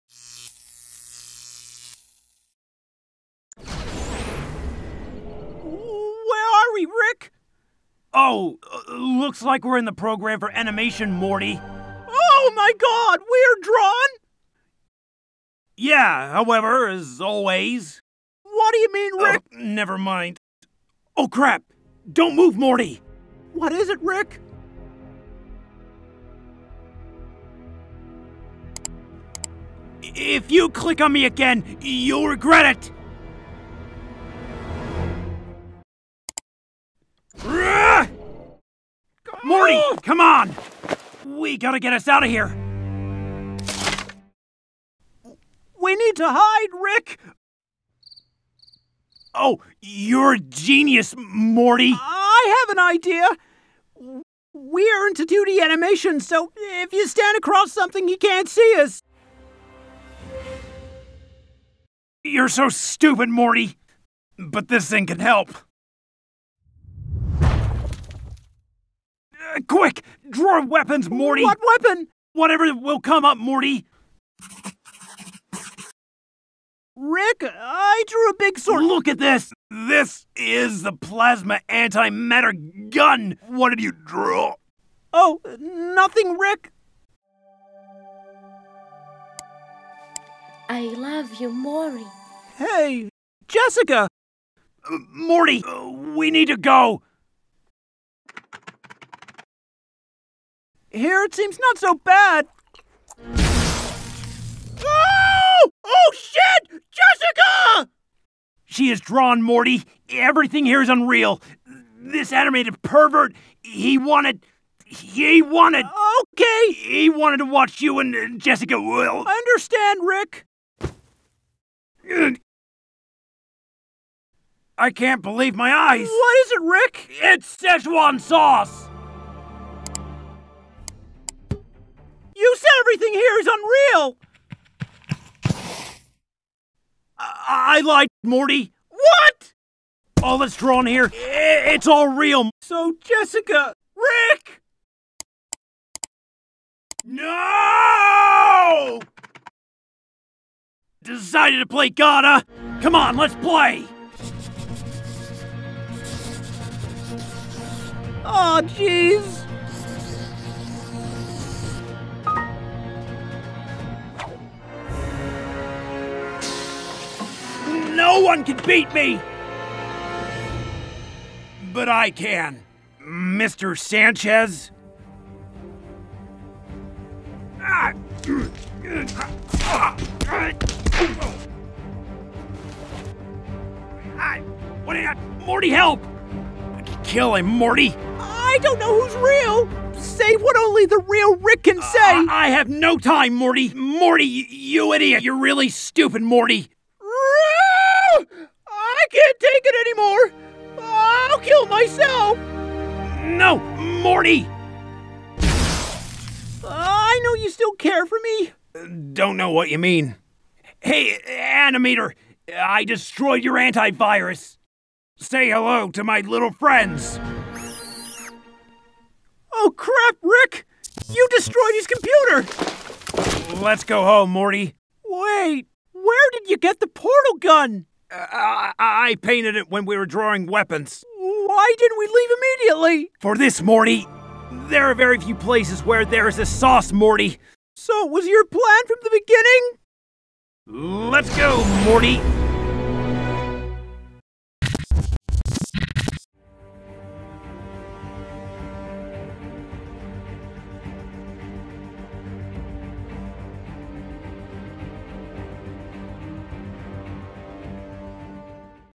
Rick And Morty vs. Animator
this animation is a parody of Rick and Morty and the animator vs animation!)
voice actor:  Mike Varker